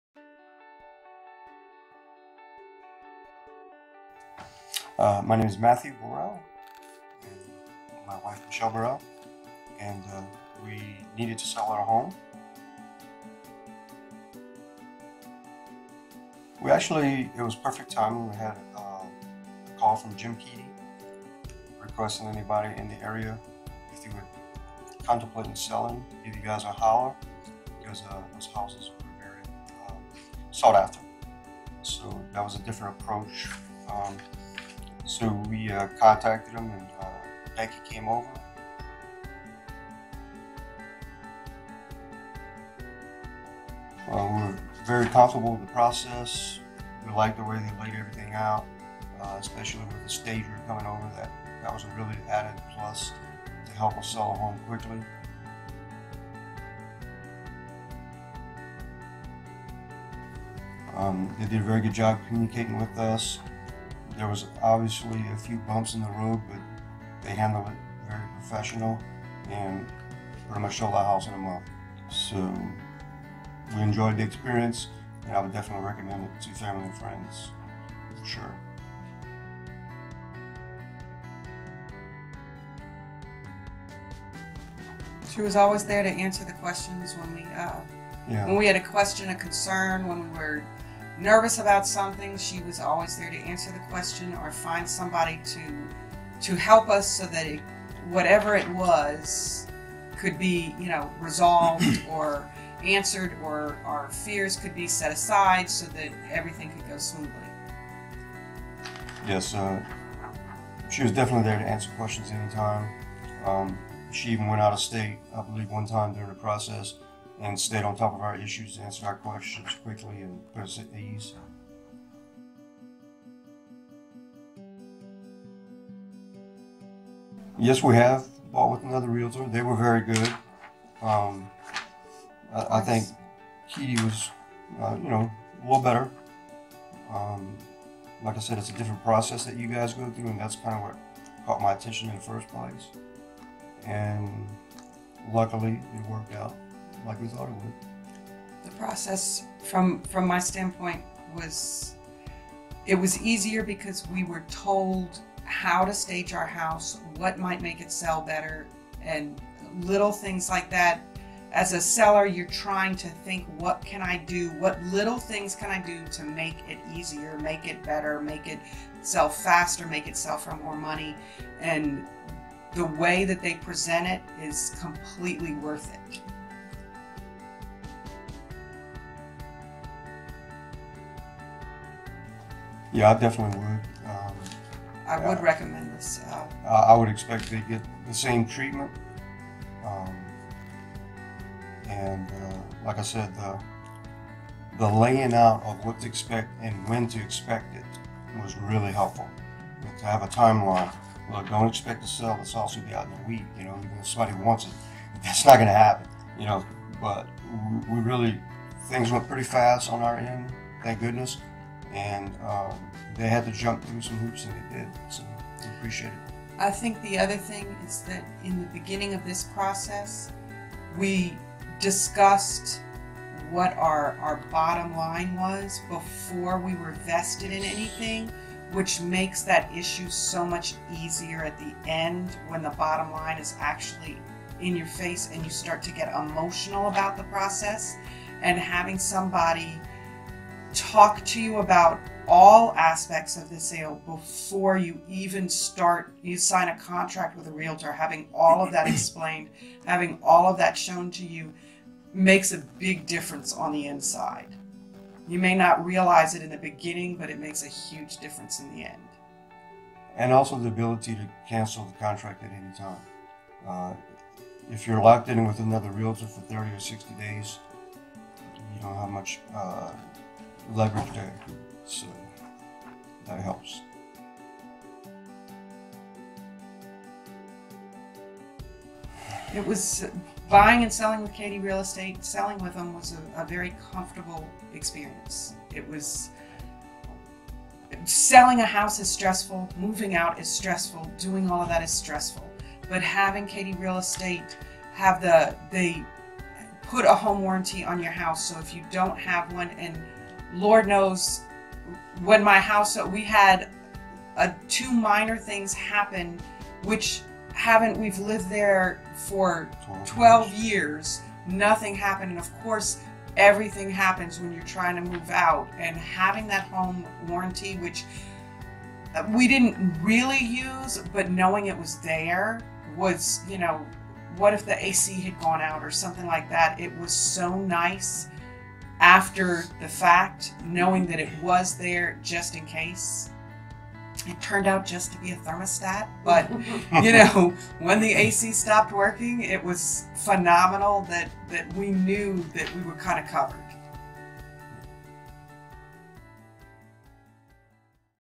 Selling with the Borel's
The Borel's were thinking about selling their home. Hear their experience and why they chose Keaty Real Estate.